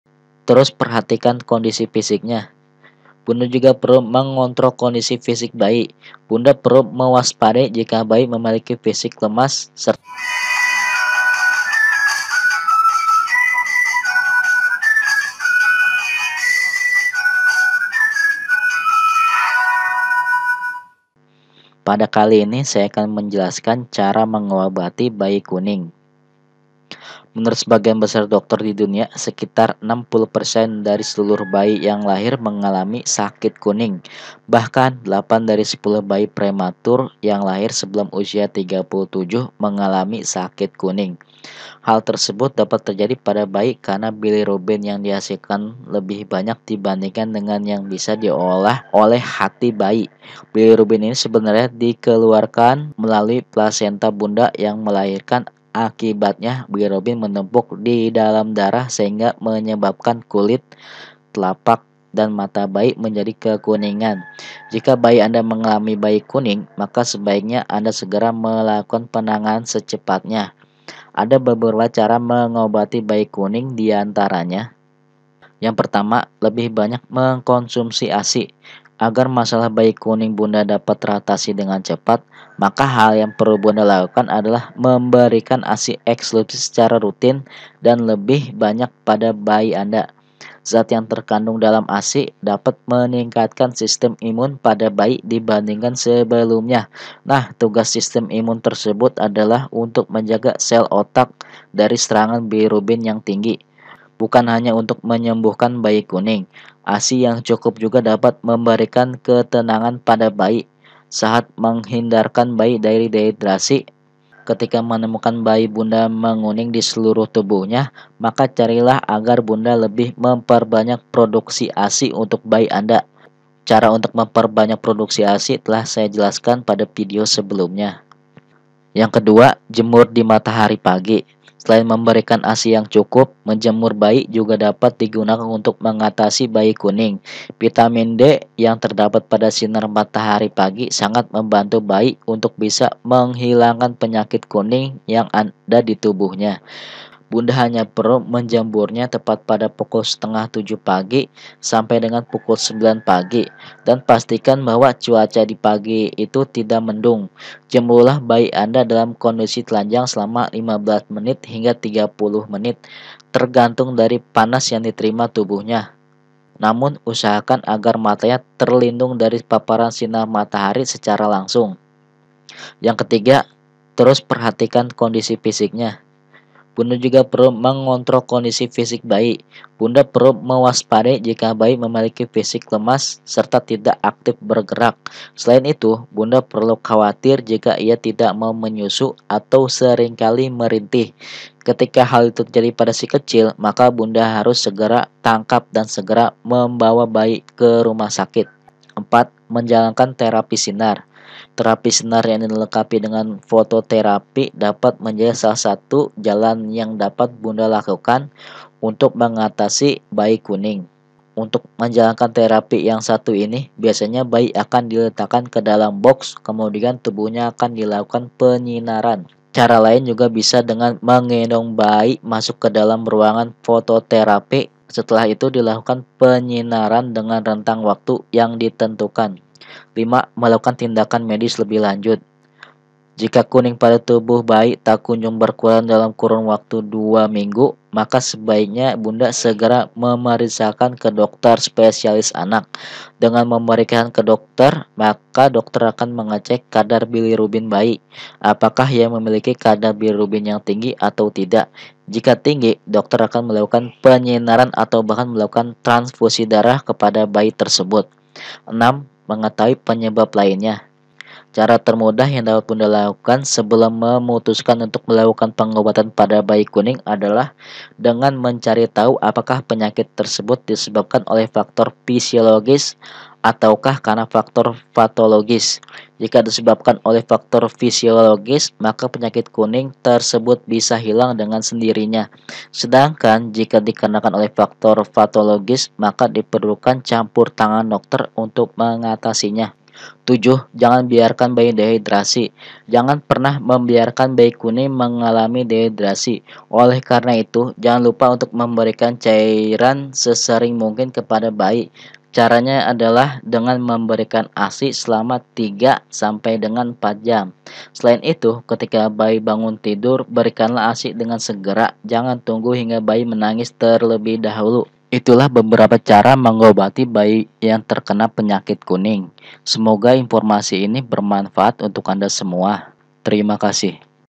7 Cara Mengobati Bayi Kuning Jika Terjadi Pada Bayi Anda SECARA ALAMI INSHALLAH SEMBUH
Mengobati penyakit bayi kuning yang perlu anda tahu setelah anda lakukan inshaallah sembuh. penyakit bayi kuning merupakan penyakit yang sering dialami oleh bayi. ada beberapa penyebab bayi kuning sehingga bayi anda terserang peyakit kuning. pada kali ini saya akan share beberapa cara mengobati bayi kuning secara alami semoga bermanfaat.